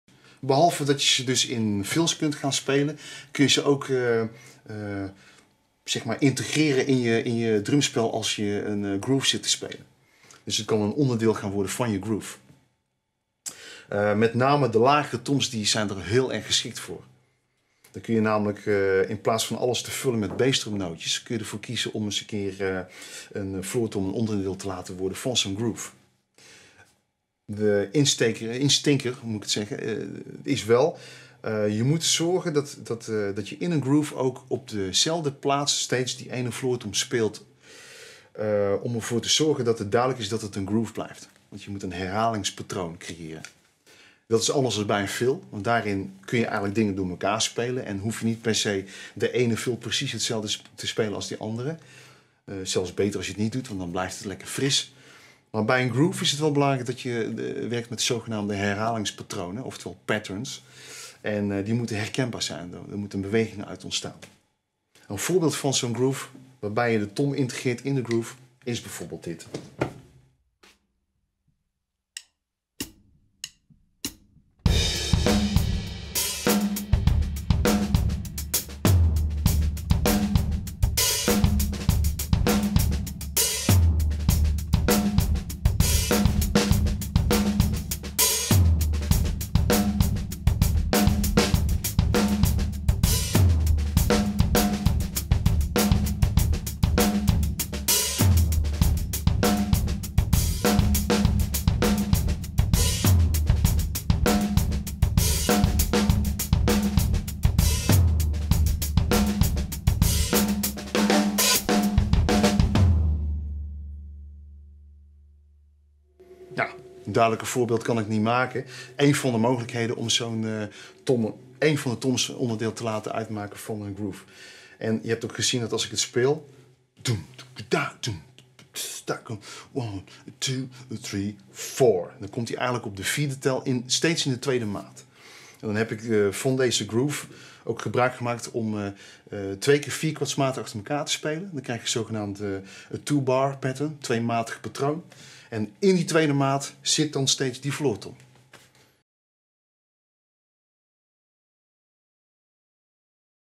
Drumtechniek Les 11, Tom Groove | Bax Music
In deze videoserie behandeld Juan de diverse 'potten en pannen' van het drumstel. Kick, snare, hihat, bekkens, toms... hij bespreekt elk onderdeel en laat meteen zien en horen wat de juiste speeltechniek is!